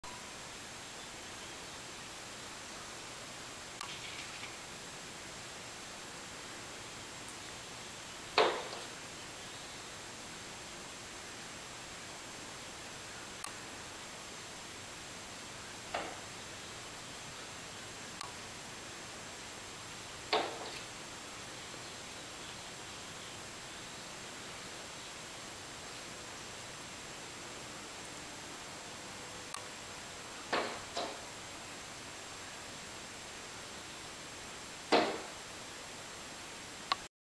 Luna watches bird attack
As our mystery bird attacks again and again, Luna displays her best posture and most alert expression, but takes no action to protect her home from winged creatures.